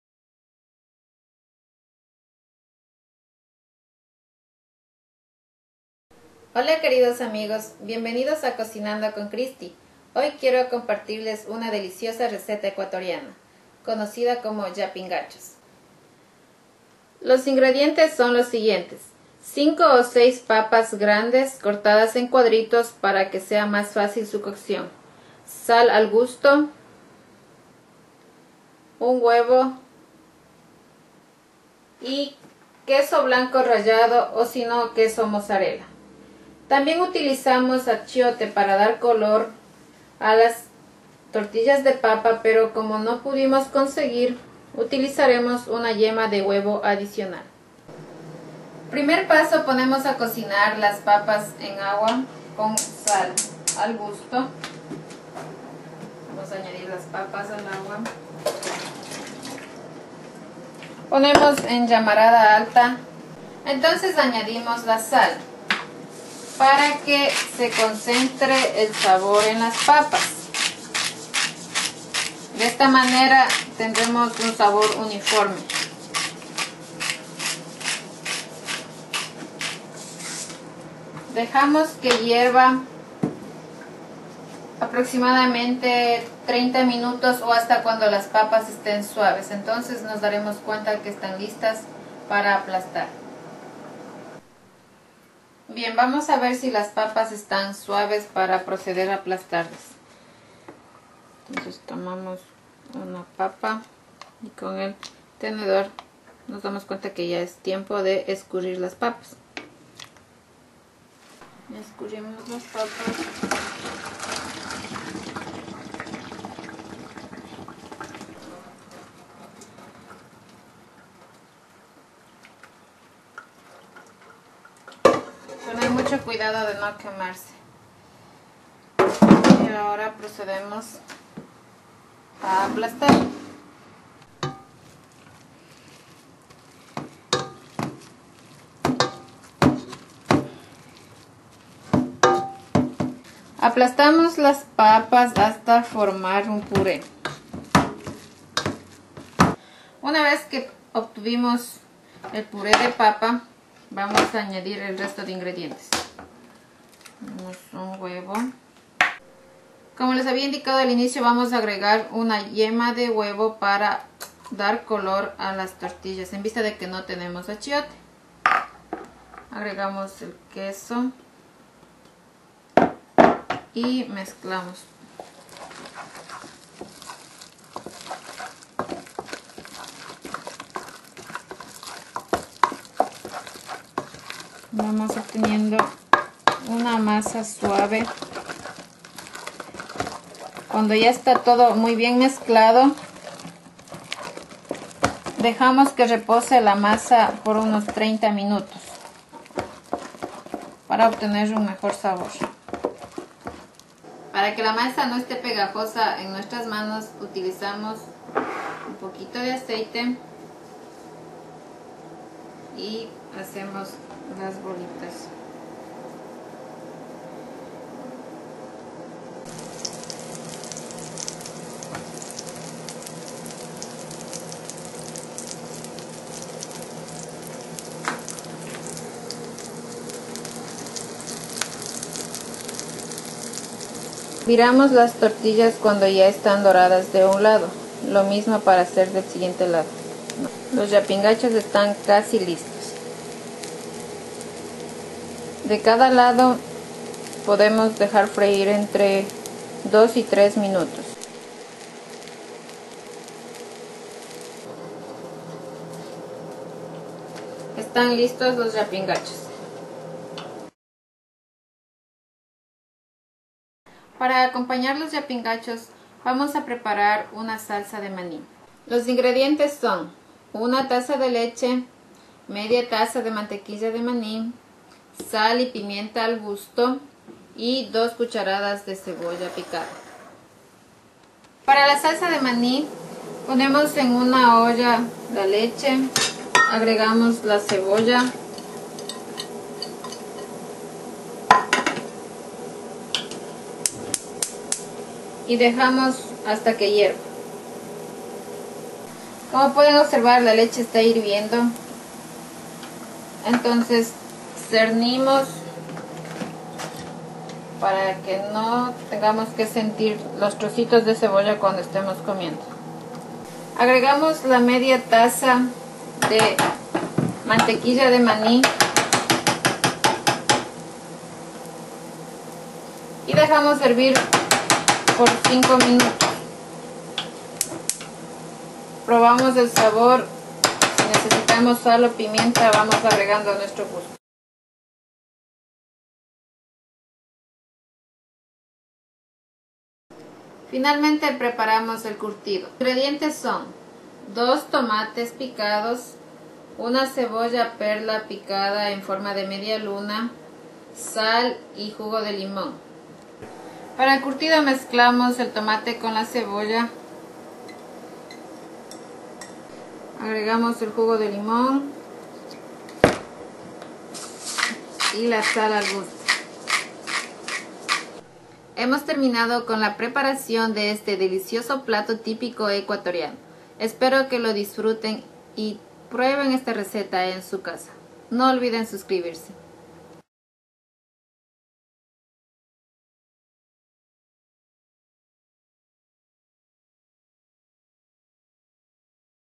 Cómo Cocinar Llapingachos con Salsa de Maní Cocinando Con Cristy
Cómo Cocinar Llapingachos Cocinando Con Cristy

Ingredientes para 12 llapingachos
6 papas grandes cortadas en cuadritos
Sal al gusto
1 huevo
1 taza de queso mozarella
1 cucharita de achiote o 1 yema de huevo

Preparacion
Cocinar las papas con sal hasta que esten suaves para aplastar y formar un pure.
Añadir el huevo, el achiote y el queso al pure de papas.
Dejar reposar por media hora
Hacer las tortillas y poner a freir en un poco de aceite hasta que los llapingachos esten dorados por los dos lados.

Salsa de Mani
Ingredientes
1 taza de leche
1/2 taza  de mantequilla de mani
Sal al gusto
2 cucharas de cebolla perla picada en cuadritos

Preparacion
Hervir la leche con la cebolla picada.
Cernir la leche.
Agregar la mantequilla de mani y sal a la leche
Hervir la salsa por 5 minutos.

Curtido
Ingredientes
2 Tomates picados
1 cebolla perla picada en estilo media luna
sal al gusto
jugo de limon al gusto

Preparacion
Mezclar la cebolla y el tomate
Agregar la sal y limon al gusto.

Poner los llapingachos en un plato, cubrirlos con la salsa de mani y el curtido.